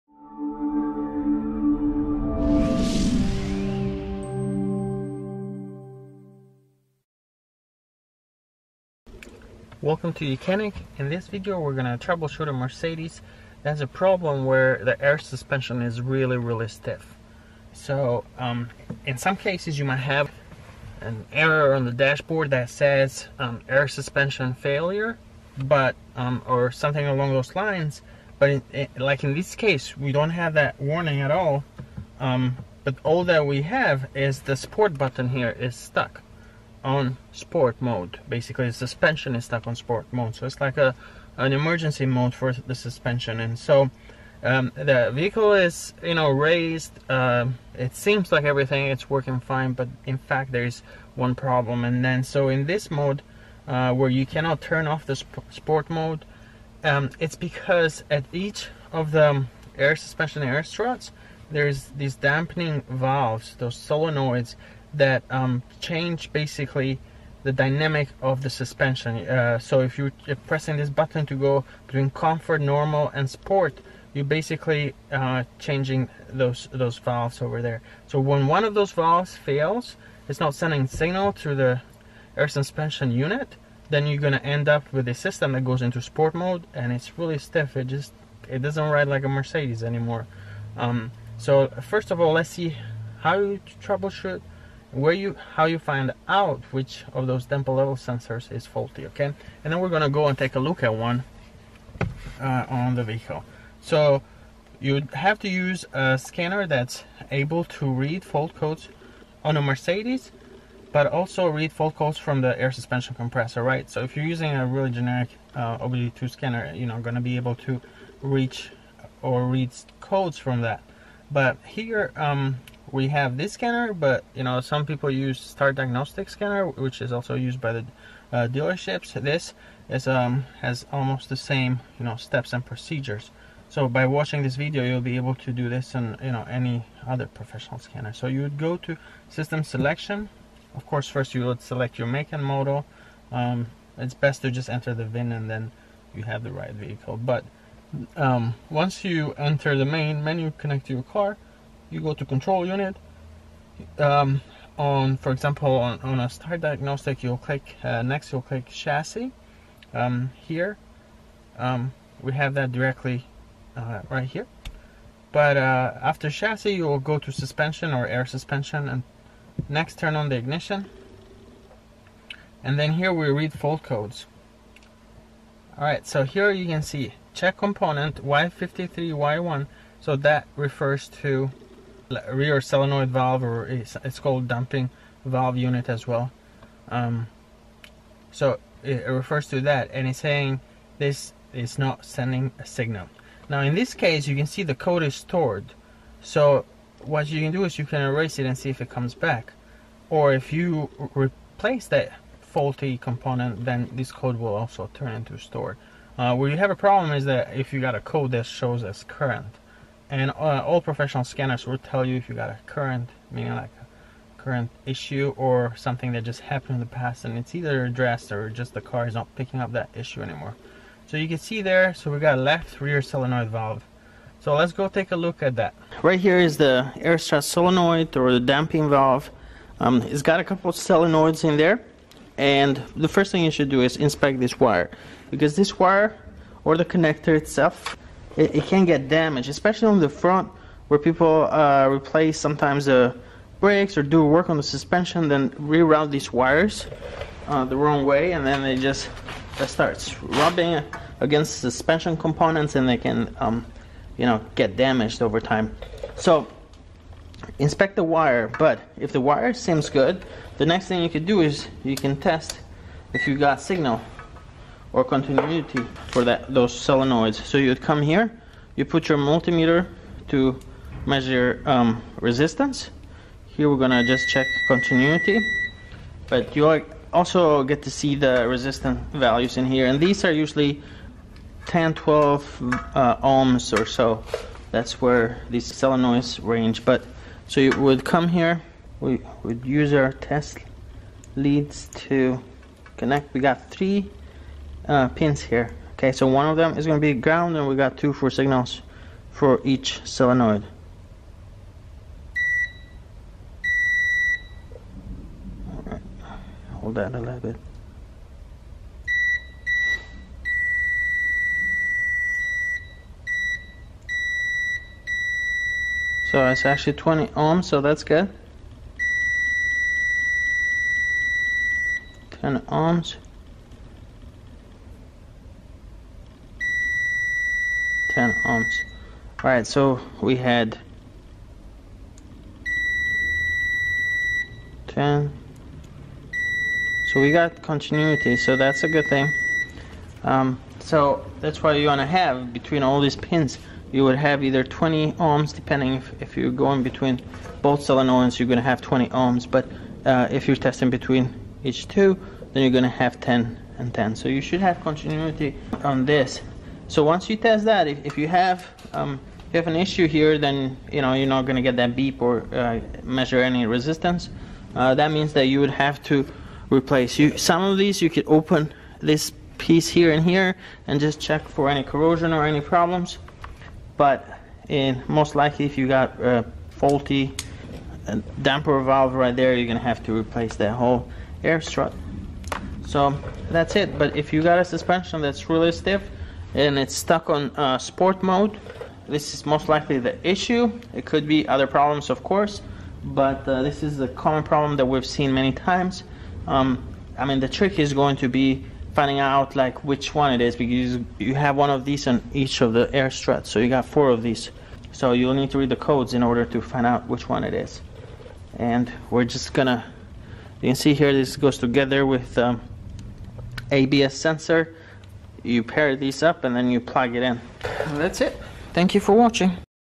Troubleshooting Mercedes-Benz Air Suspension Electrical Problems
This video offers a comprehensive guide on troubleshooting electrical issues related to the air suspension system on a Mercedes-Benz vehicle. The video demonstrates the tools and equipment needed and the necessary steps to diagnose common electrical problems, such as faulty sensors or wiring. The video also provides tips and tricks on fixing or replacing defective parts and preventing potential issues from arising in the future. The video intends to help Mercedes-Benz owners and enthusiasts save money by identifying and fixing electrical problems related to their vehicle's air suspension system rather than taking their vehicle to a mechanic or dealership. By following the instructions in the video, car owners can ensure that their vehicle's air suspension system is functioning properly, which can improve ride comfort and handling and prevent potential safety issues on the road.